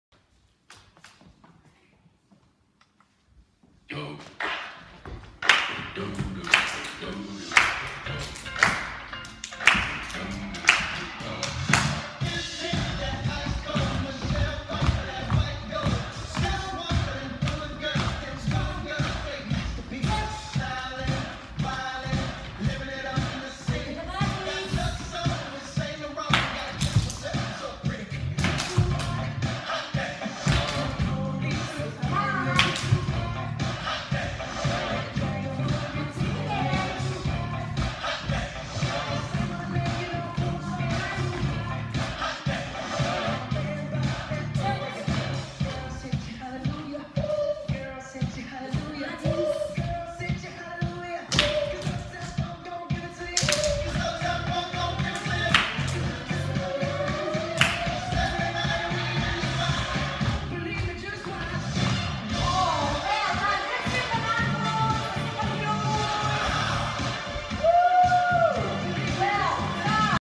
Radio Borders Superhero Stomp
Watch Fiona from Beat Pulse Dance teaching Courage the Cat how to do the Superhero Stomp for Cash for Kids Superhero Day!